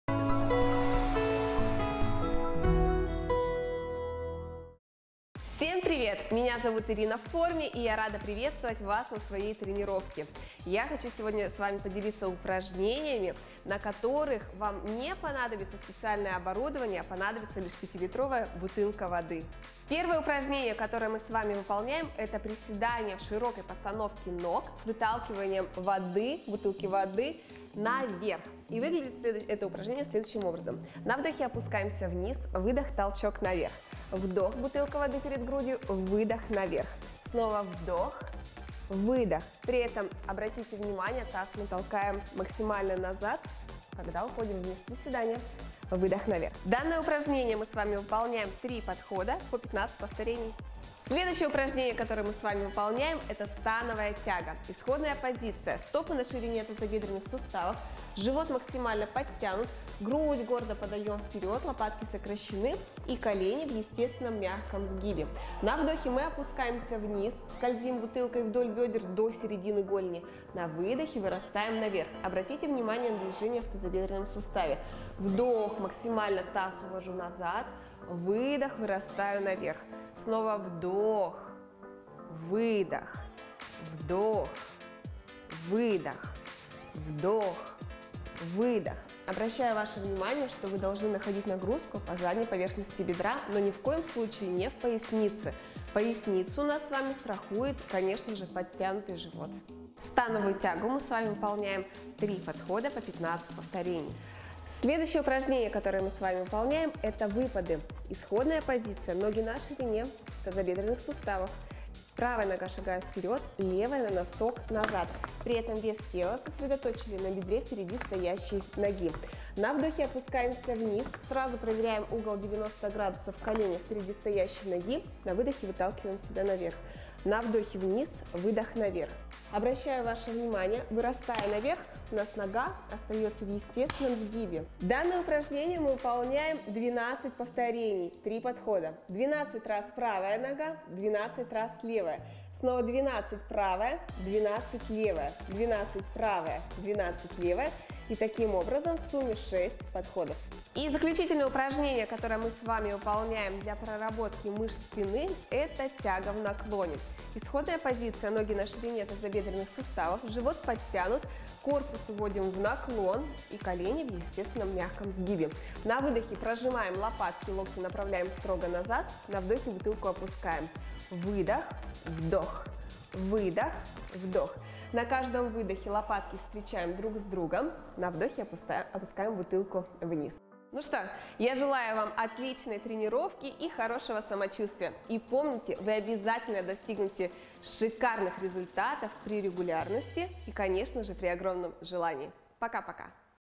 Югорчанкам показали, как укрепить спину с помощью 5-литровой бутылки воды
Рубрика «Домашняя тренировка». Автор - Эльдар Хасанов.